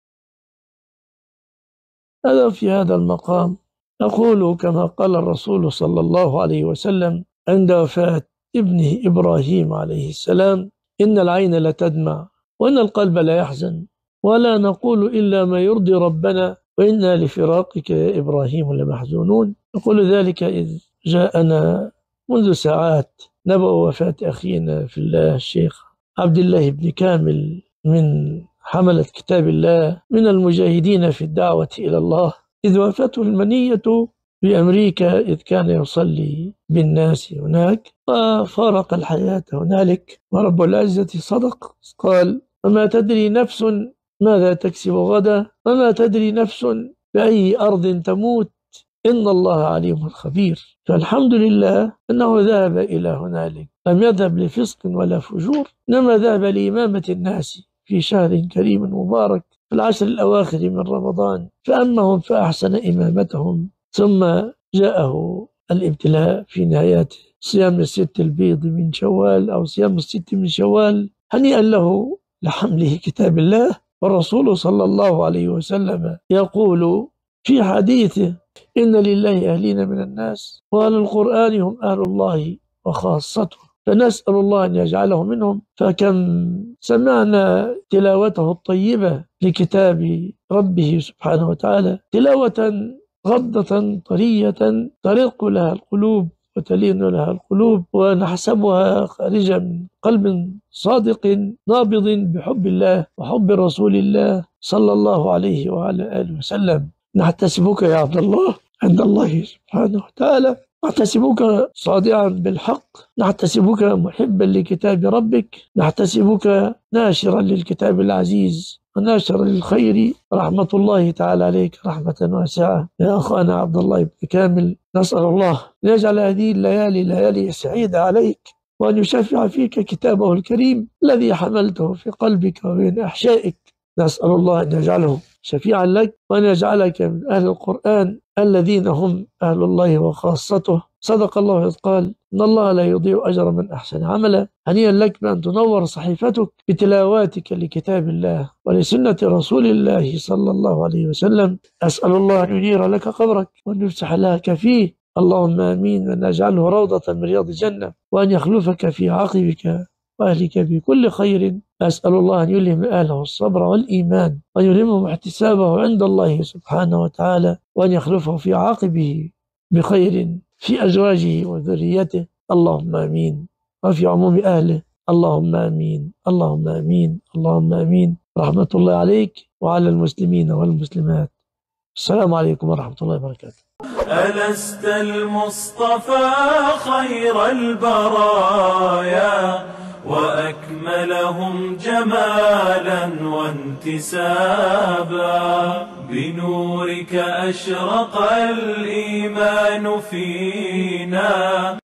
بكاء الشيخ مصطفى العدوى فى وفاة الشيخ عبدالله كامل
قناة أحباب الرسول للكتاب المسموع Ahbab Al-Rasul audiobook channel